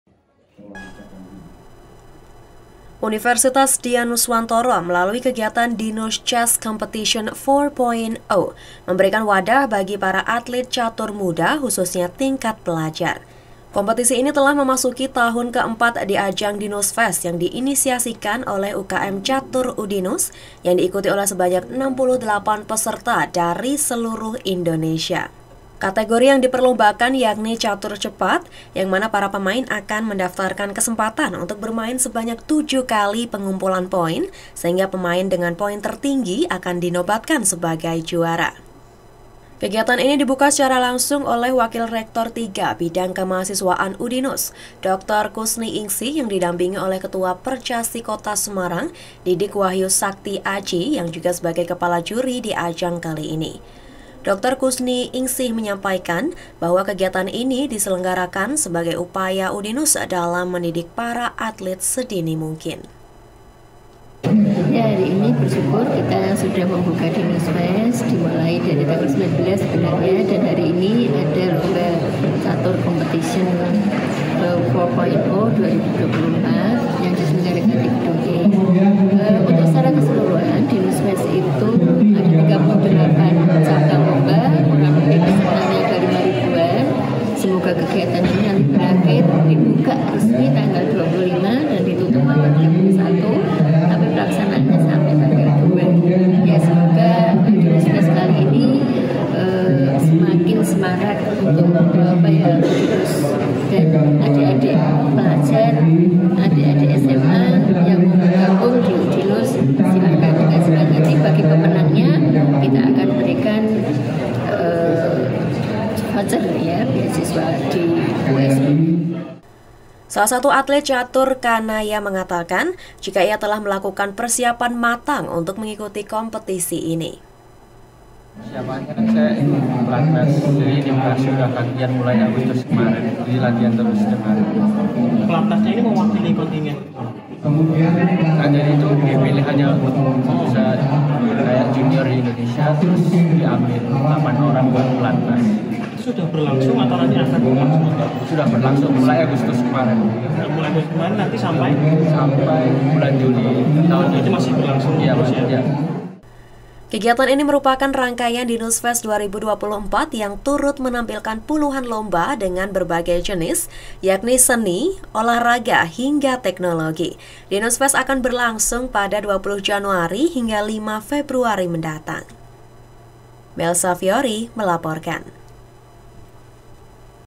Dinus Chess Competition 4.0 Ramai Diikuti Atlet Catur Muda Se-Indonesia
Berikan wadah bagi atlet catur muda tingkat pelajar di Indonesia, UDINUS gelar dinus chess competition 4 point o. Kompetisi ini juga ditujukan guna menggali bibit unggul bagi dunia catur.  (mel)